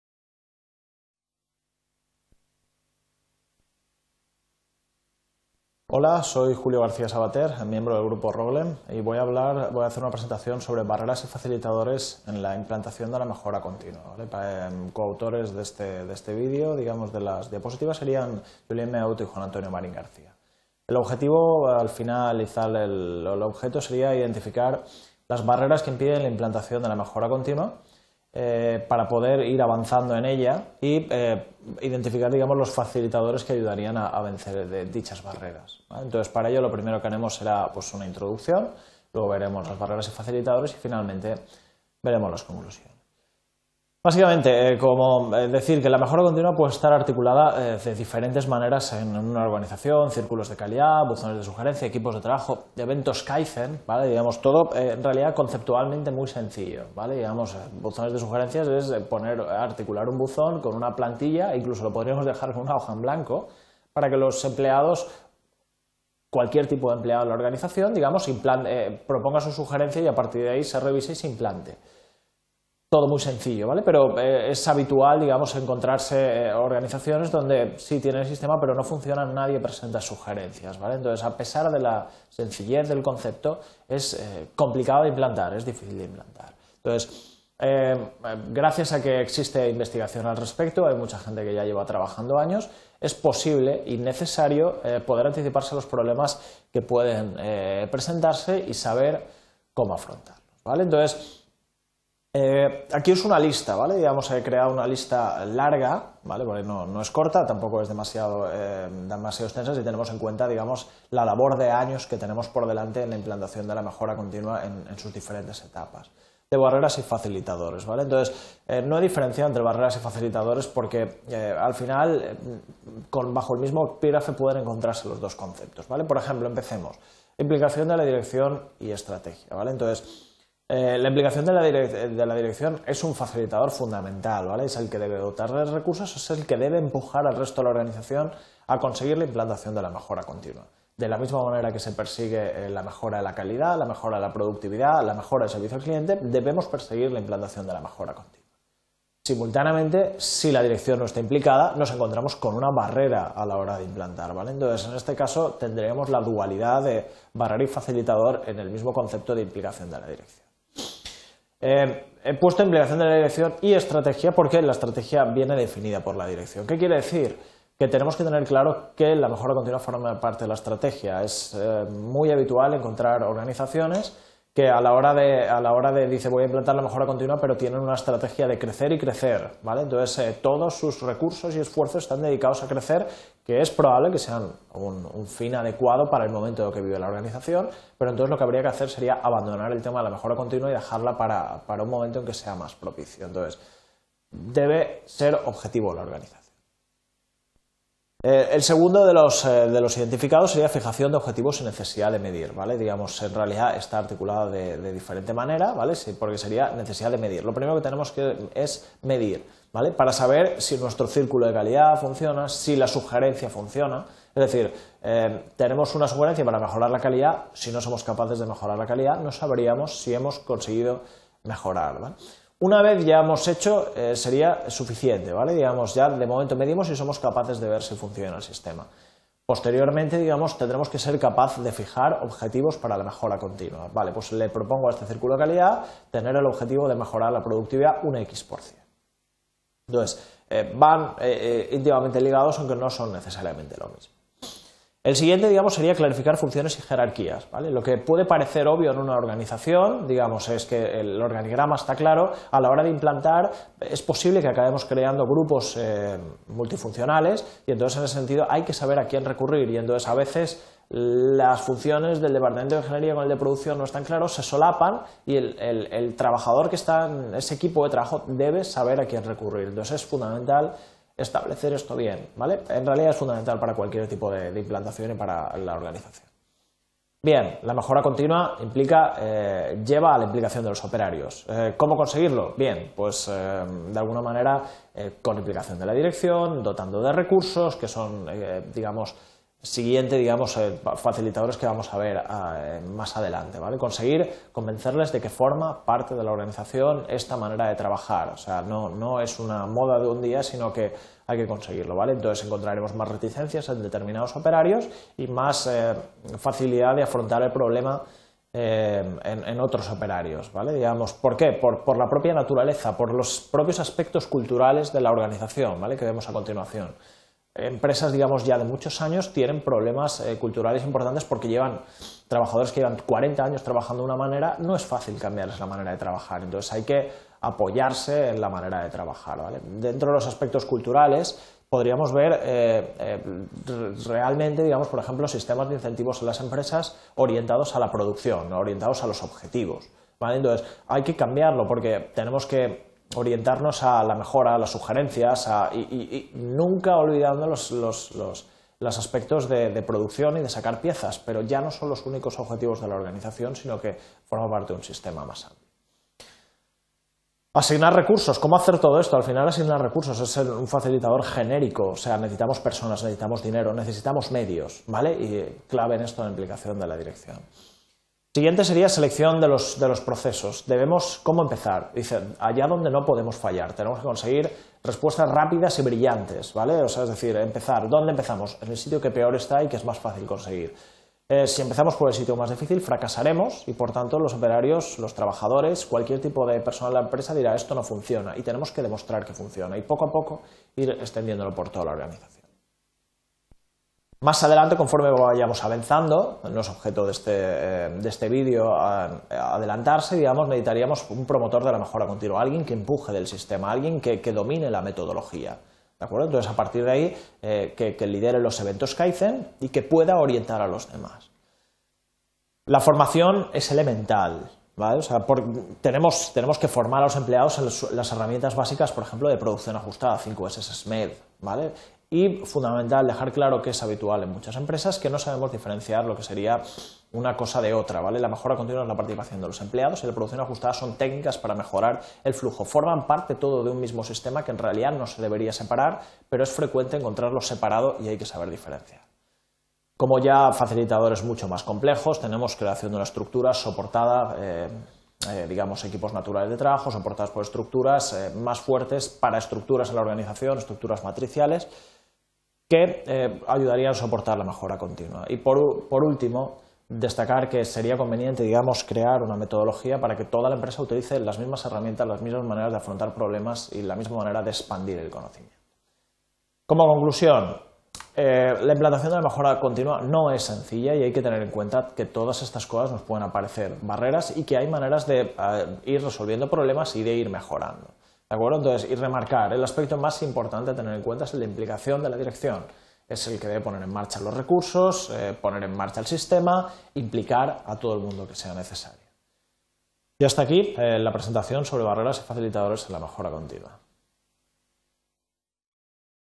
Barreras y facilitadores en la mejora continua| UPV
Título: Barreras y facilitadores en la mejora continua

Descripción: El vídeo presenta una descripción de las barreras y facilitadores existentes en las organizaciones y que intervienen en el éxito o fracaso de la implantación de sistemas de mejora continua García Sabater, JJ.; Maheut, JPD.; Marín García, JA. (2012). Barreras y facilitadores en la mejora continua.

Autor/a: García Sabater Julio Juan